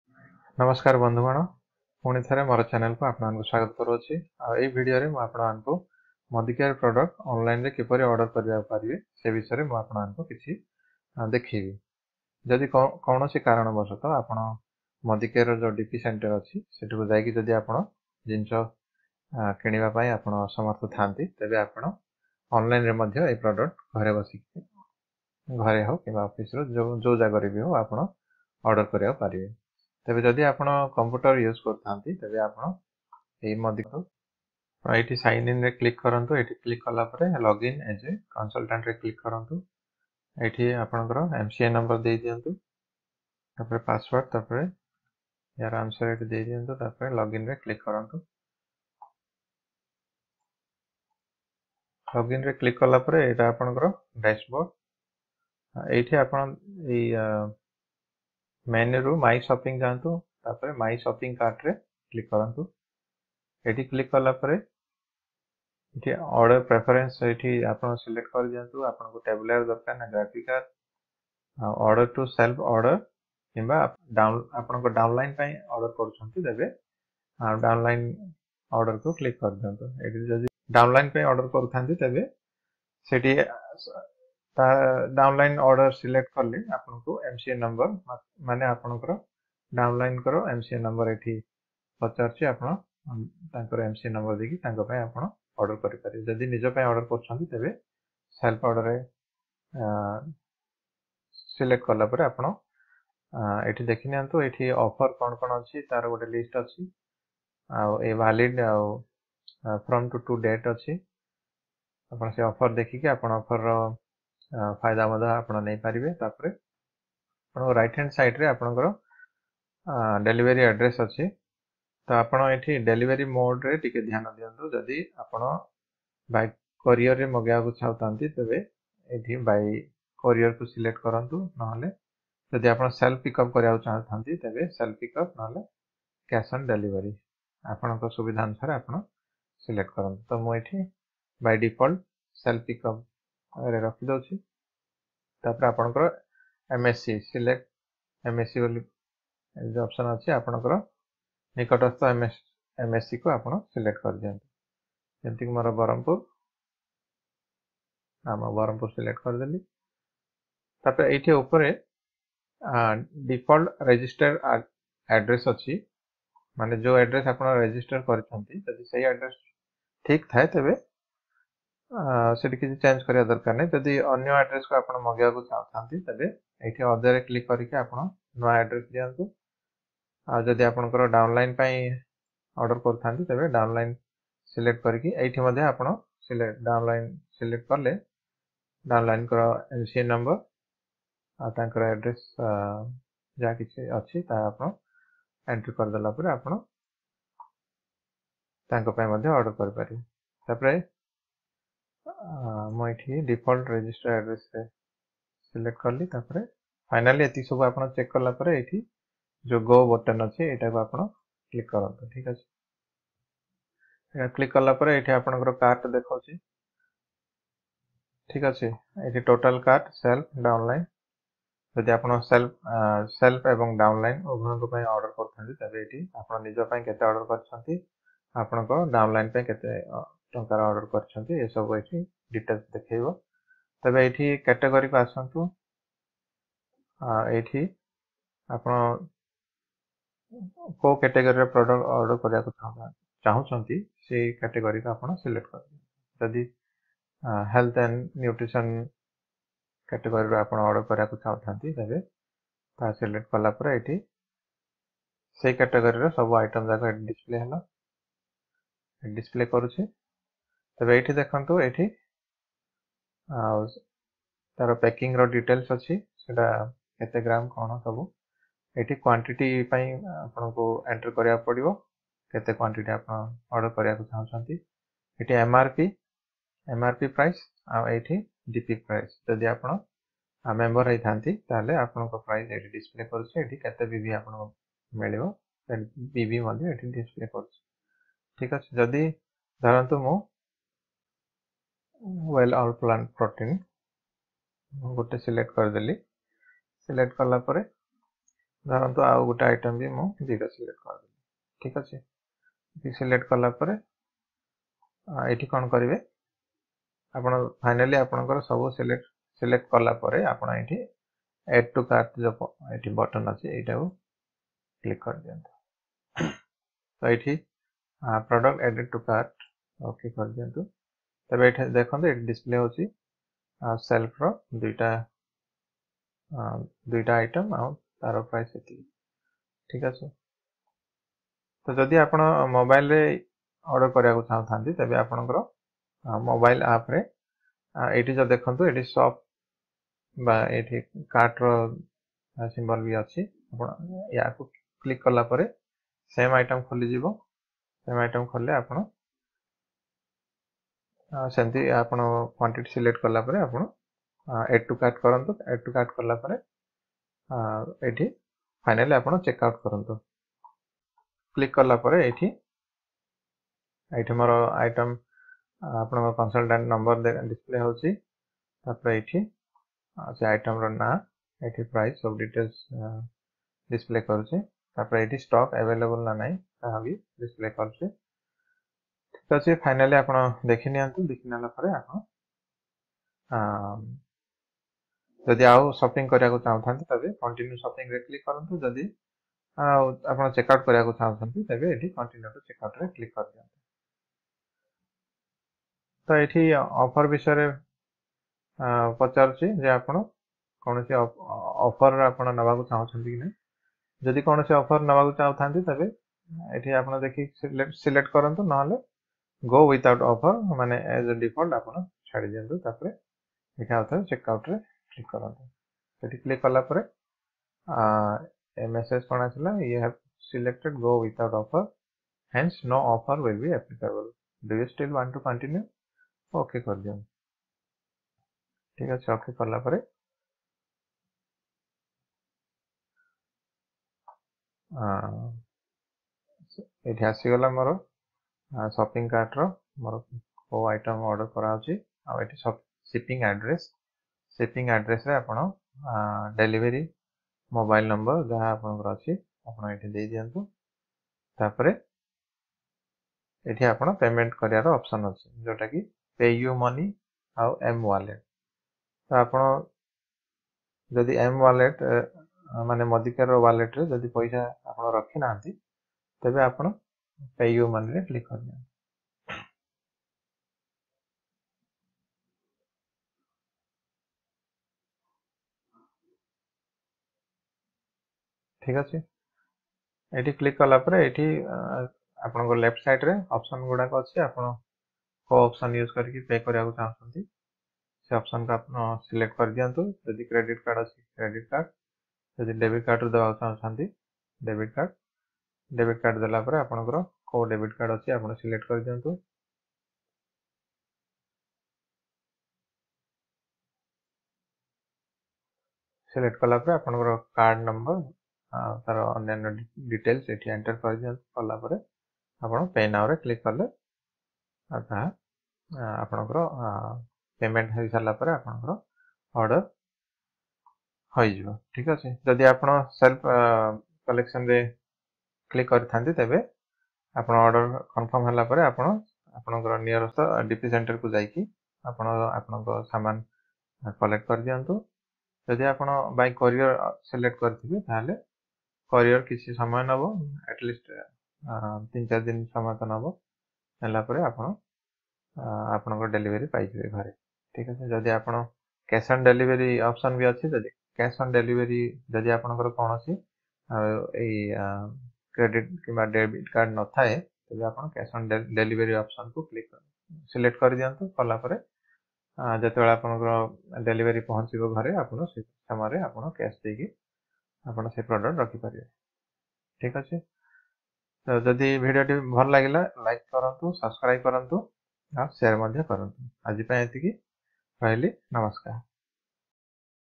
How to Order Modicare Product Online in Odia Modicare-Product-Order-Online
If you would like to order Modicare products online then please follow the steps explained in this video.

The steps are easy to follow and if you have any experience to purchase online then you might skip this video and you can try yourself.

Facebook Messenger @ m.me/singh.rajratna1